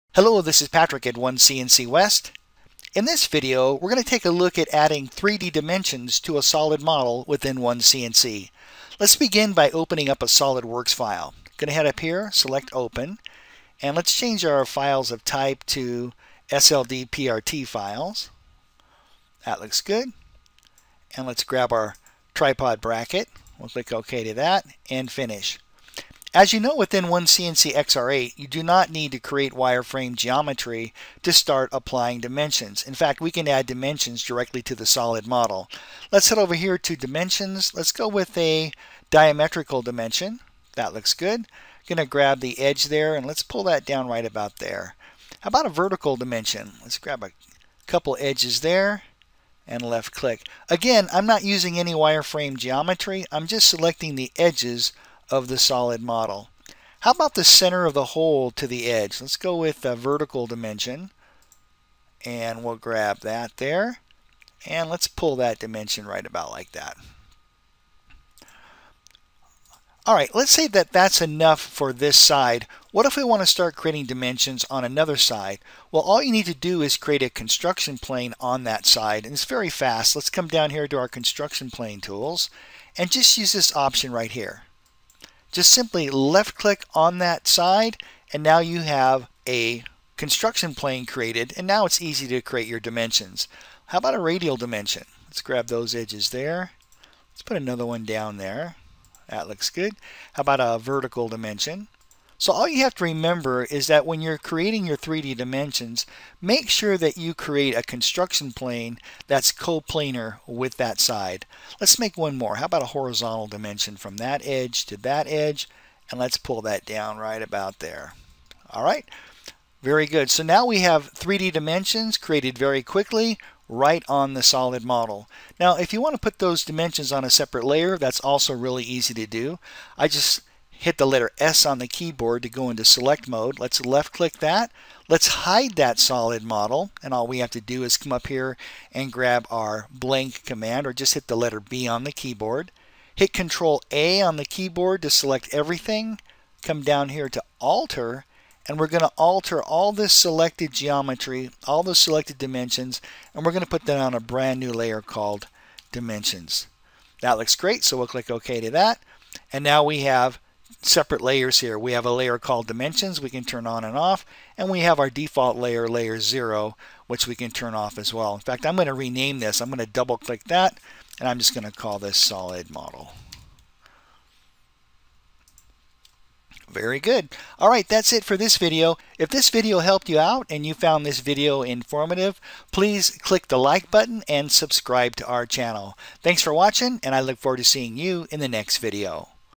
OneCNC CAD/CAM: 3D Dimensions
OneCNC CAD/CAM: 3D Dimensions Explained

In this video you will learn how easy it is to add 3D dimensions using OneCNC. There is no need for wire frame geometry as dimensions can be placed directly on the solid model.

You will also learn how to quickly use constructions planes to assist with your 3D dimensions.